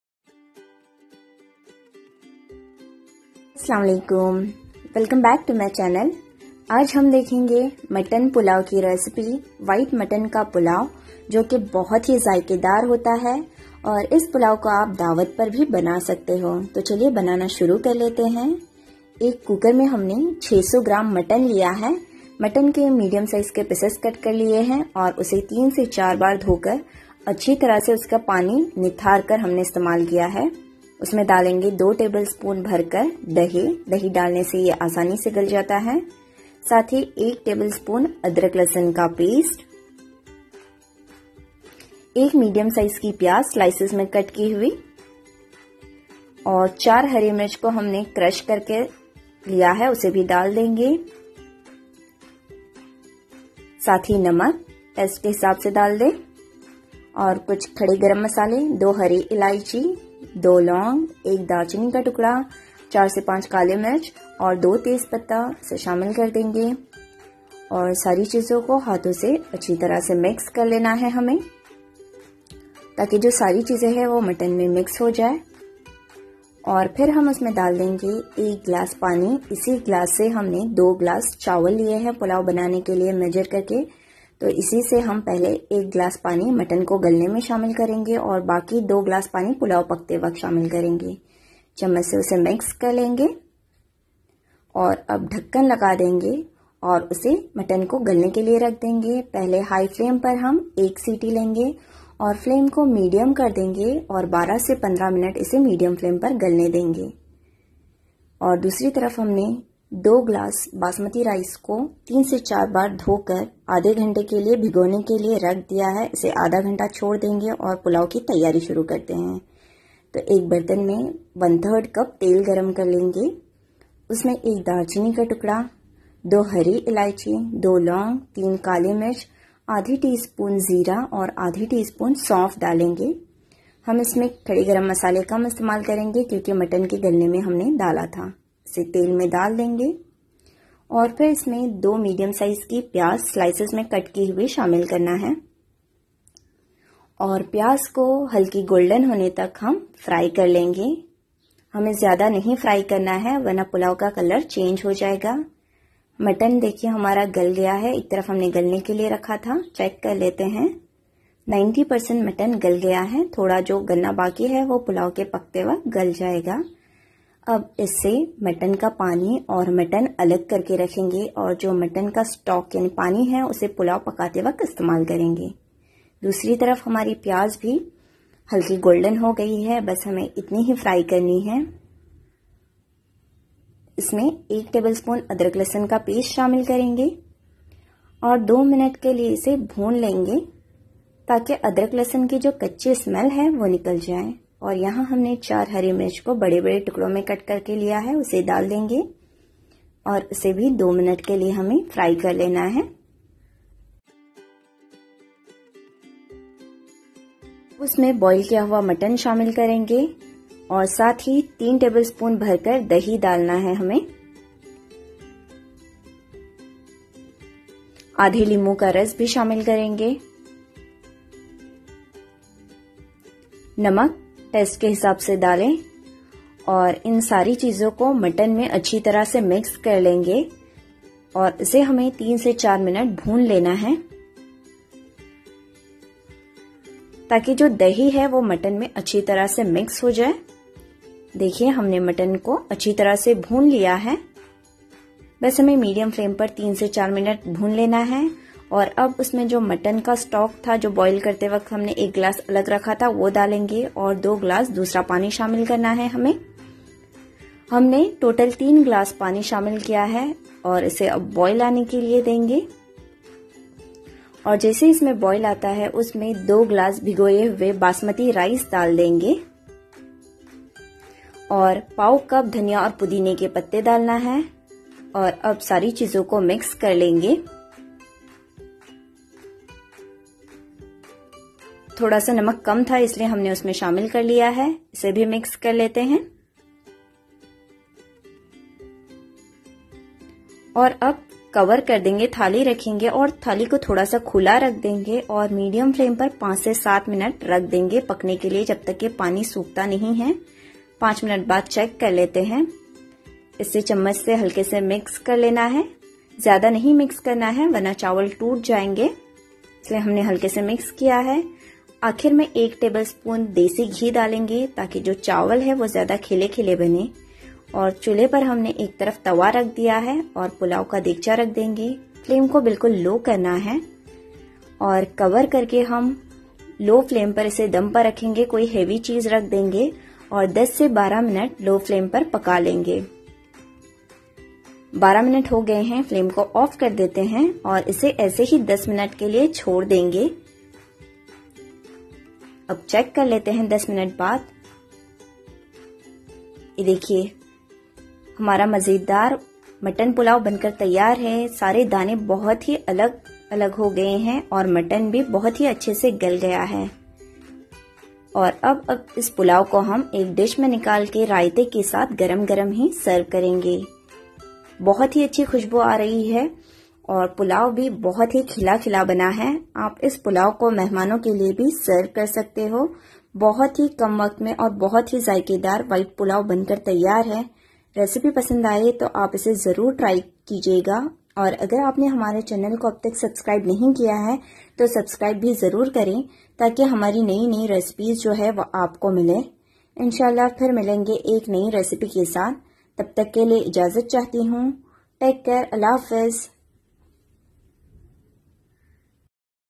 White Mutton Pulao | Mutton Pulao Recipe
Ingredients:

Green Chilli Crushed 4
Onion-1
Salt to taste
Pepper 5
Clove 2
Cinnamon stick 1
Cardamom 2
Bayleaf 2

Basmati Rice 400 gm
Oil 1/3 cup
Onion 2
Cardamom 2
Cinnamon stick 1
Clove 2
Pepper 3
Lemon 1/2
( 1 glass Mutton stock + 2 glass water = 3 glass water)
Coriander Leaves and mint leaves 1/4 cup